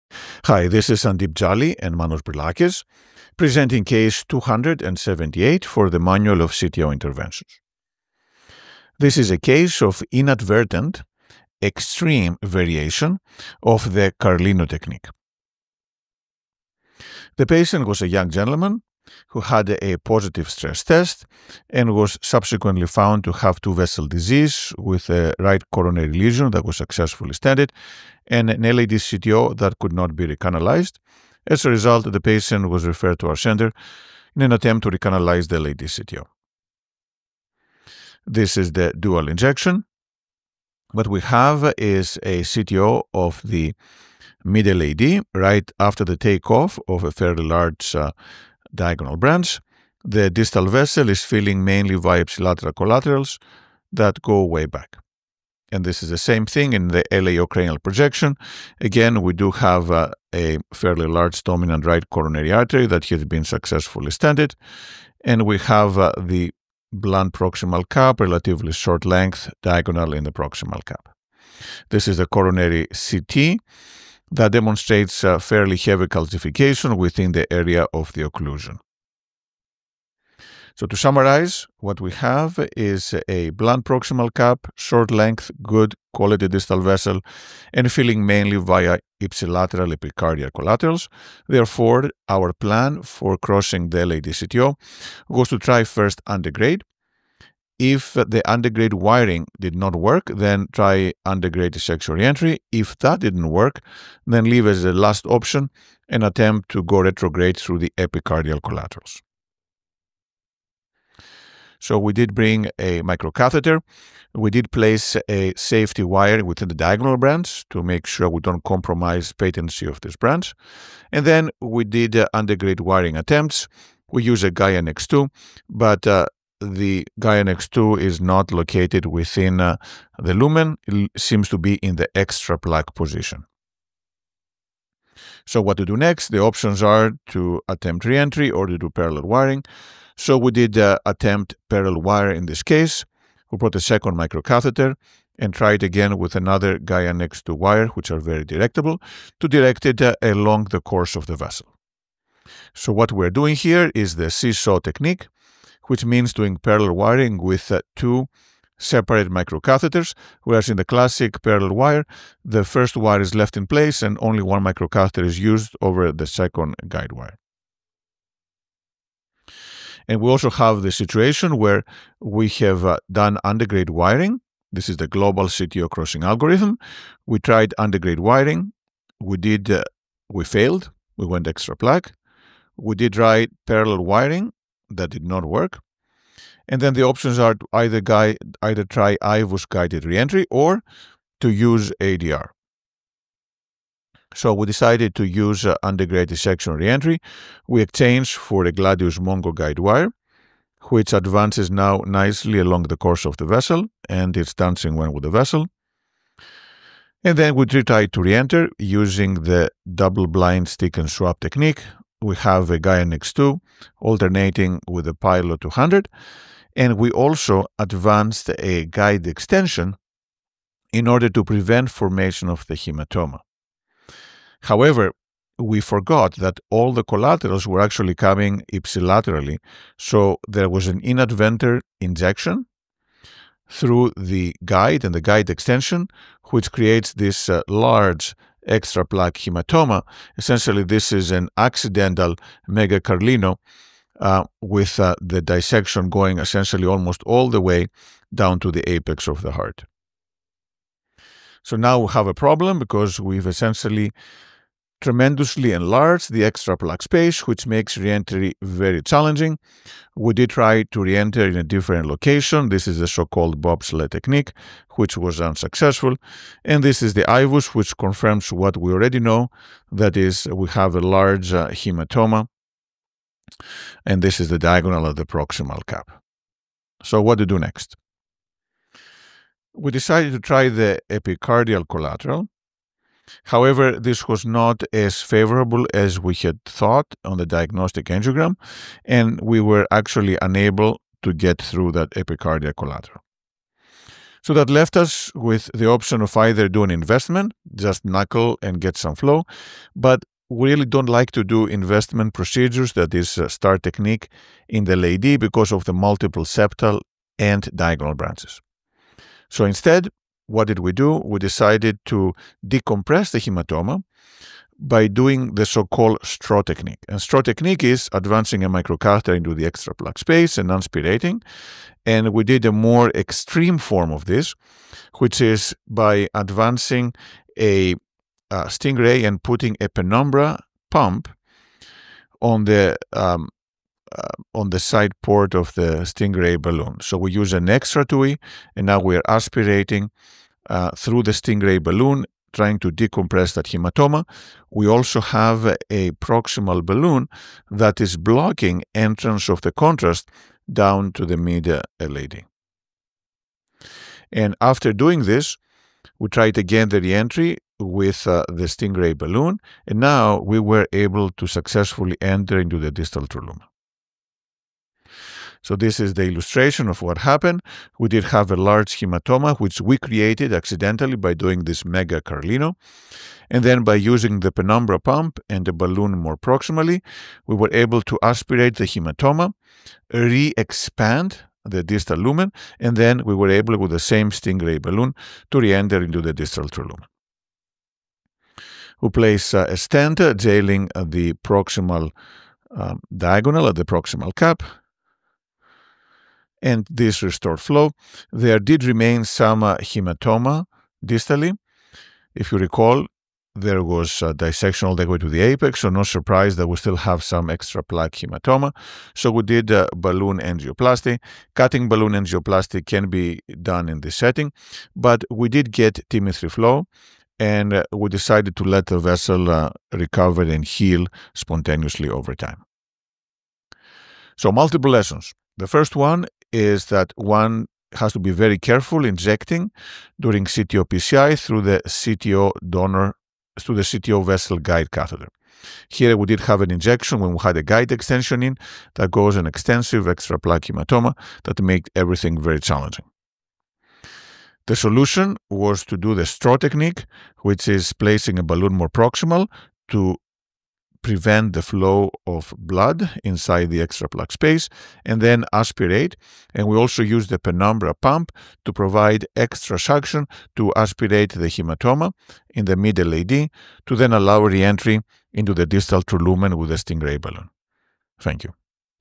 Case 278: Manual of CTO PCI - Mega Carlino
A patient was referred for PCI of a mid LAD CTO after a prior failed attempt. The LAD CTO had a blunt proximal cap at the takeoff of a large diagonal branch, length of approximately 20 mm with significant calcification based on coronary CT angiography, good quality distal vessel and distal filling via ipsilateral epicardial collaterals. Antegrade wiring with a Gaia Next 2 resulted in extraplaque crossing. Parallel wiring failed. We attempted re-entry with a Stingray balloon and a guide extension to block antegrade flow without success. An inadvertent injection (Mega Carlino) caused a large extraplaque hematoma extending to the distal LAD. Retrograde crossing via epicardial collaterals failed. We performed the STRAW technique (subintimal transcatheter withdrawal) to aspirate the extraplaque hematoma: a balloon was inflated proximally followed by aspiration through the Stingray balloon using a Penumbra pump. This facilitated successful re-entry into the distal true lumen using the Stingray balloon with a nice result after stenting. There remained mild extraplaque distal hematoma that was treated with balloon angioplasty.